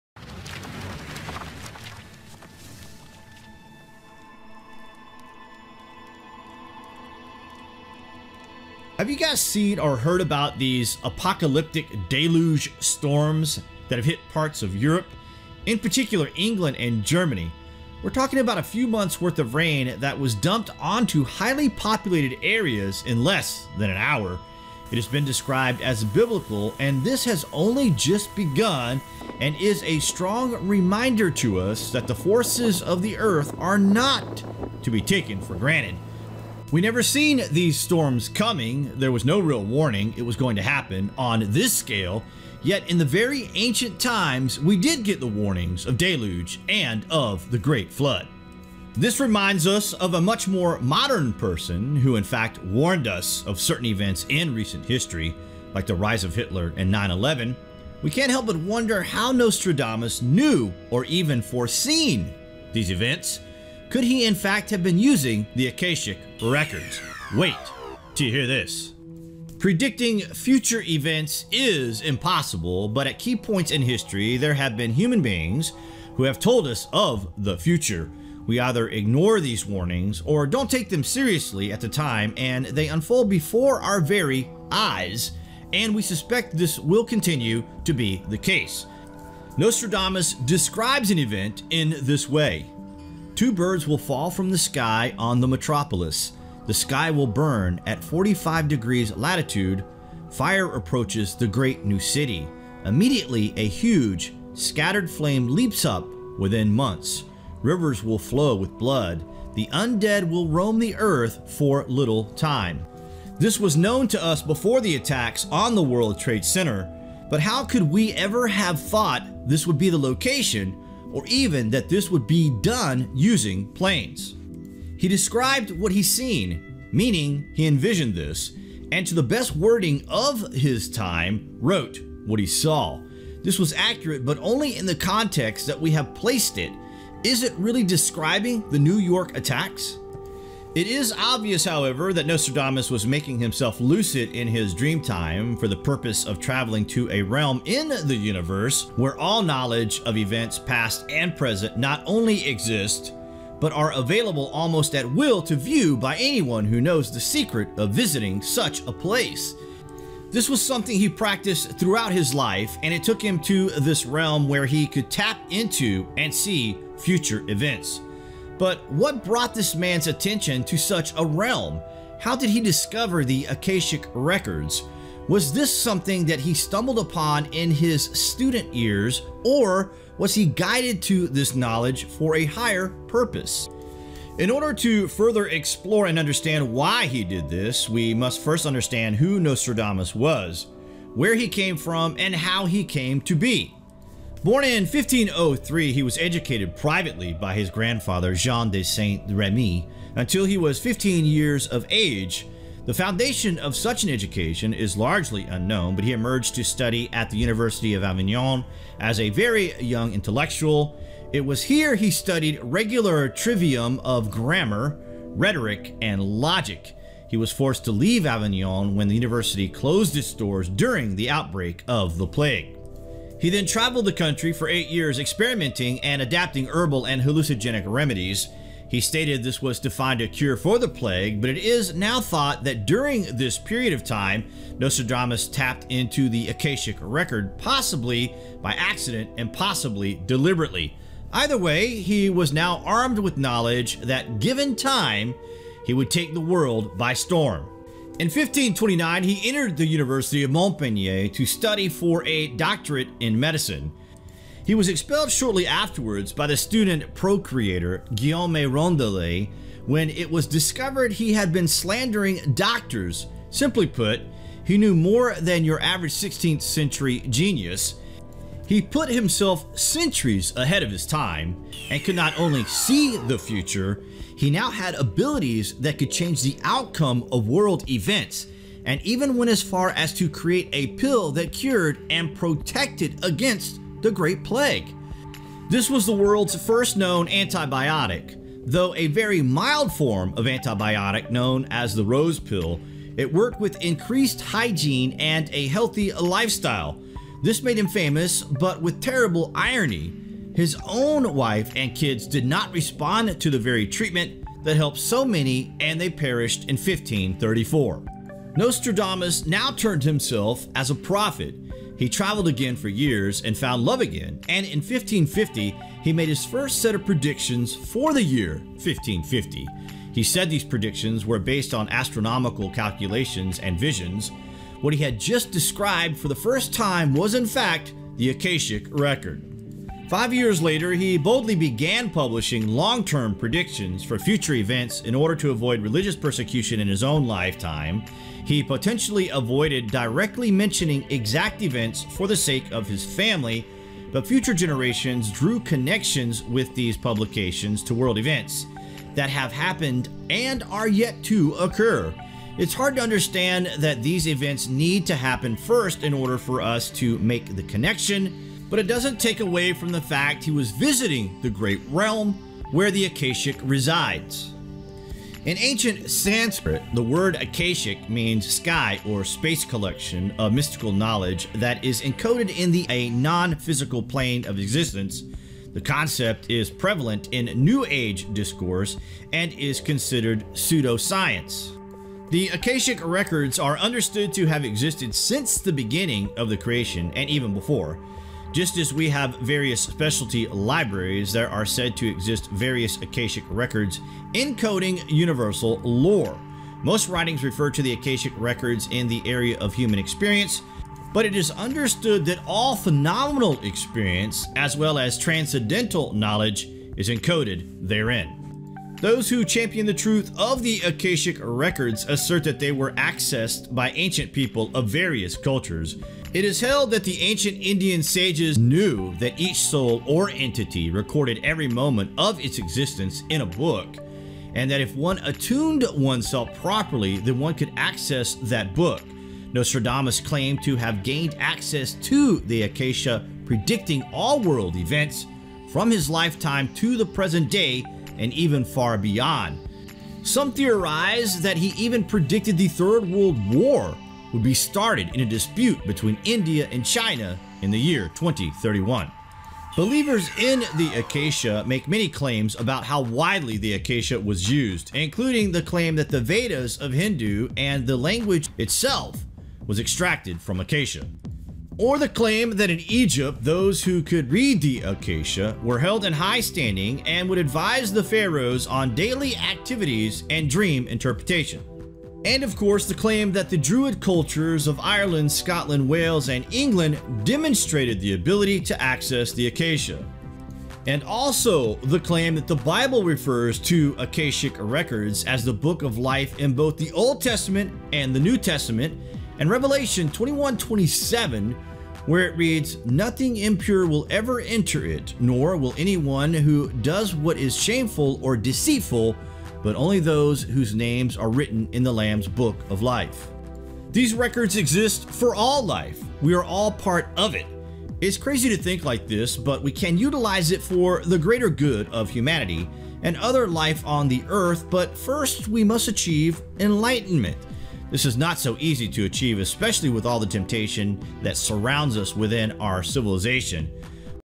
Nostradamus and the Akashic records: The Great Realm
It is almost impossible that Nostradamus relied on guess work to provide future predictions, it is far more plausible that he discovered the Akashic record and foreseen future events, what do you guys think?

Links and sources:
Footage from History with thanks:

“The Nations of Old Re-emerge from the East, a great conflict begins and the word of Man is taken from Ancient tongue, revenge is sought and the World engages” – Nostradamus.
(Possible reference to future conflict between India and China?)